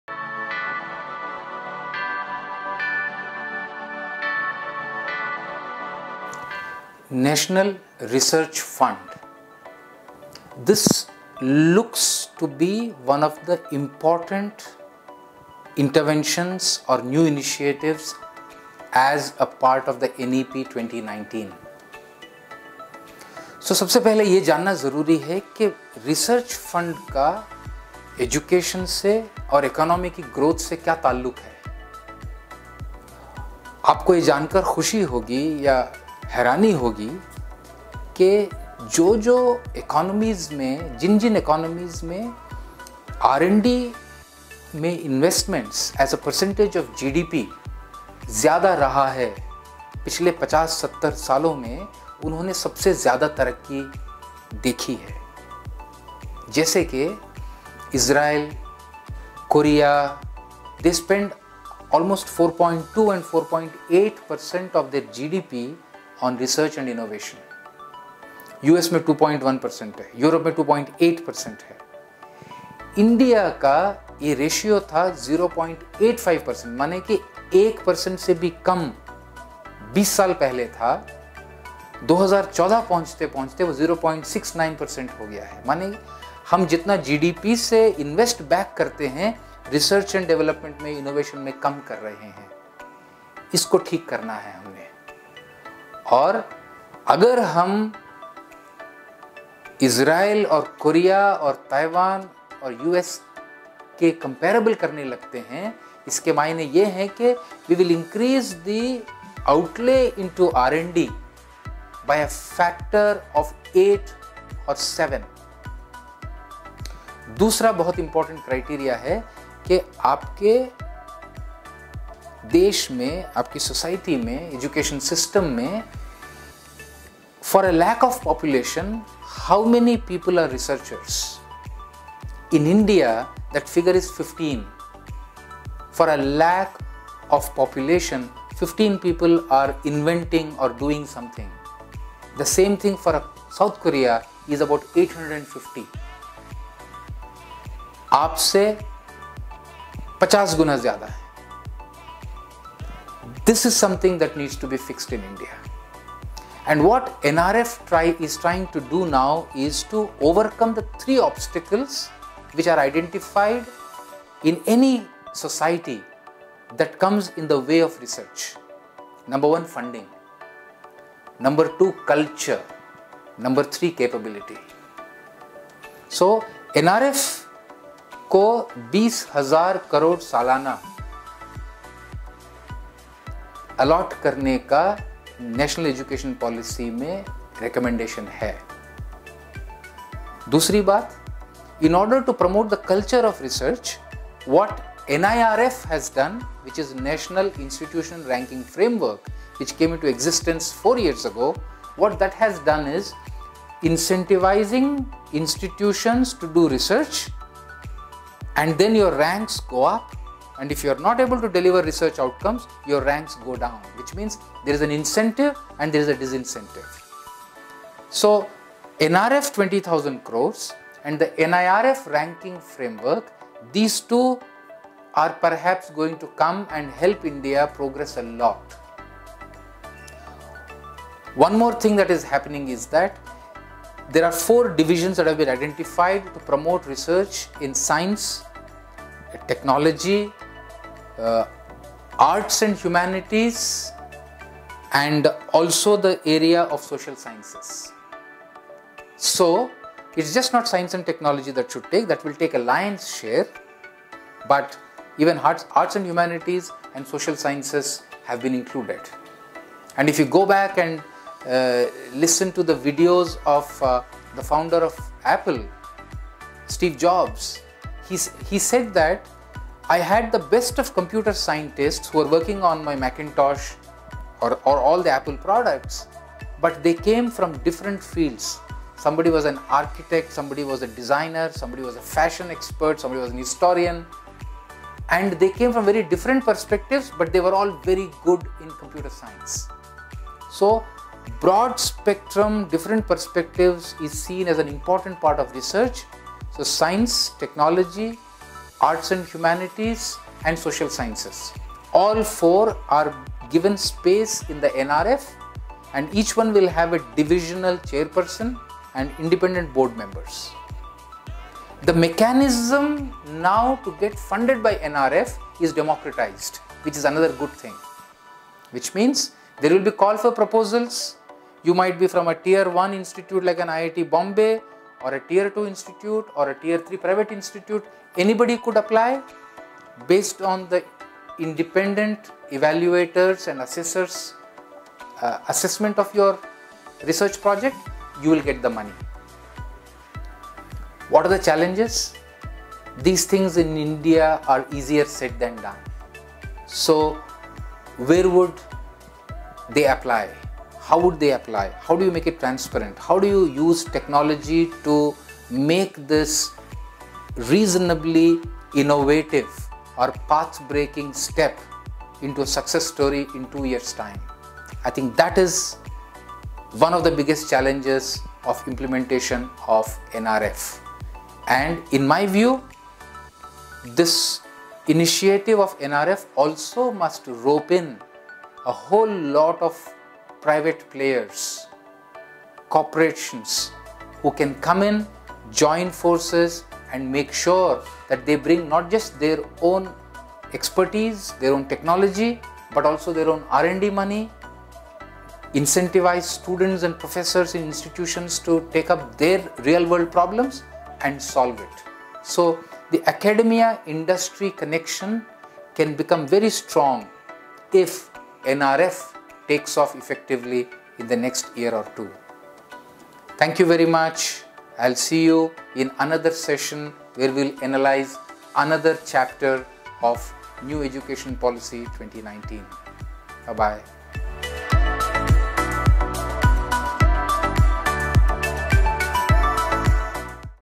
NEP 2019 || Part-2 || National Research Fund || Career Launcher.
National Education Policy 2019, an analysis by Satya Narayanan R. founder Career Launcher.